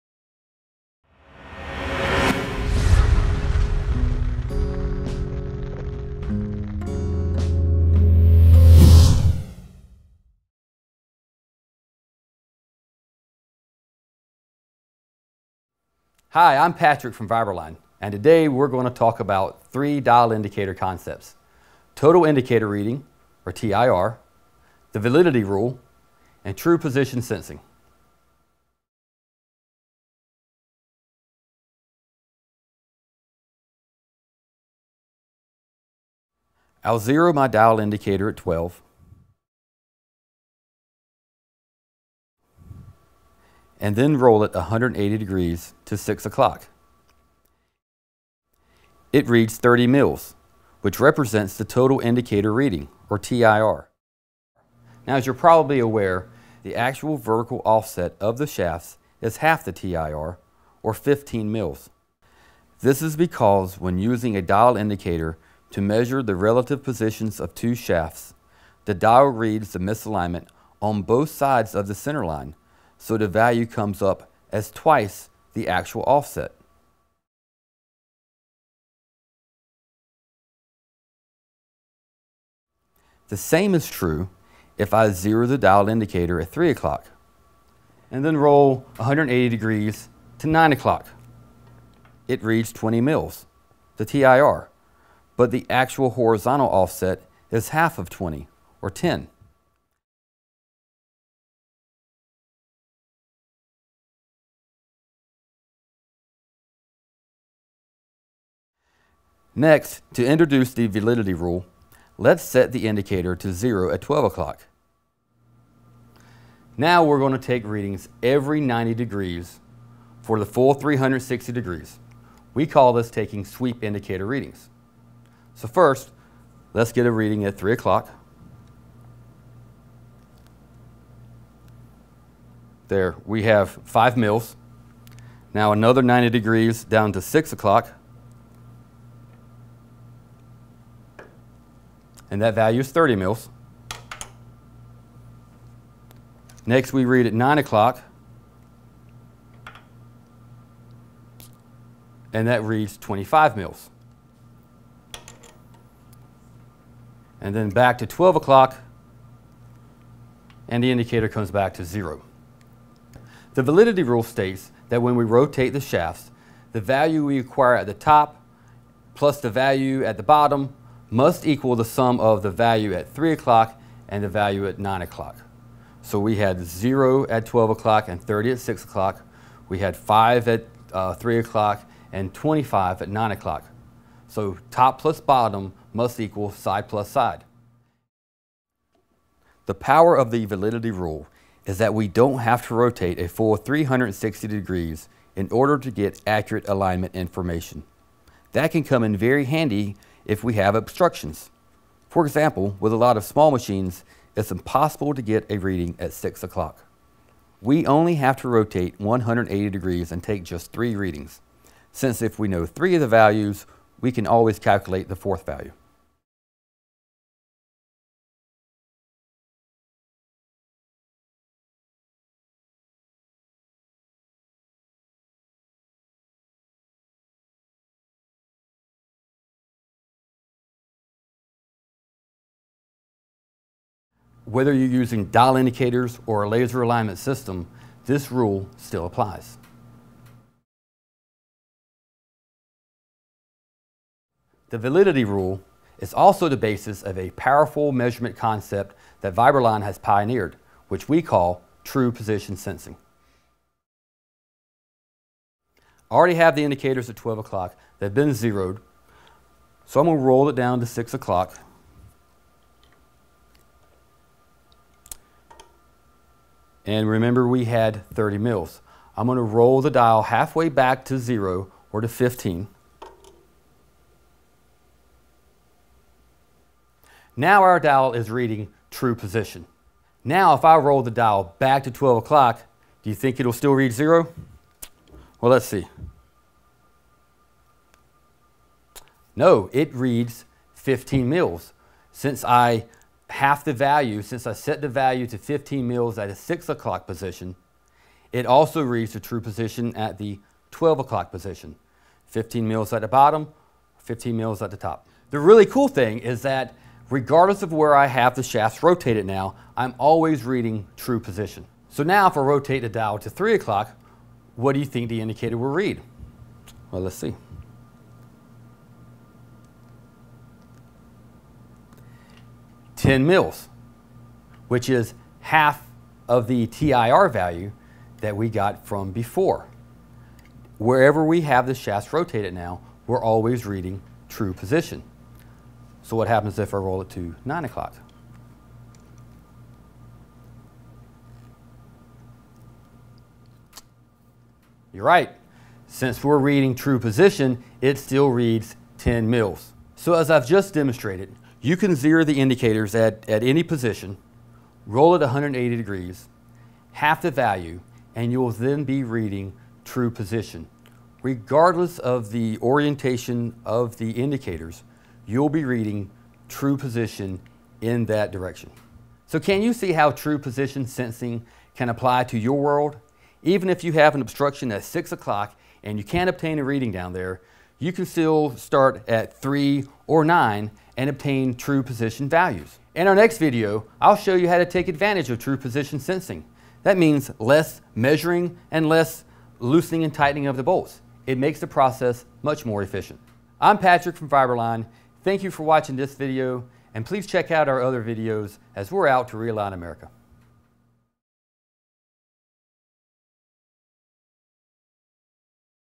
Dial Indicator Concepts: TIR, Validity Rule & TPS | ACOEM
Acoem Trainer Patrick Lawrence guides us through three shaft alignment concepts (Total Indicator Reading, The Validity Rule, and True Position Sensing) with the help of a dial indicator.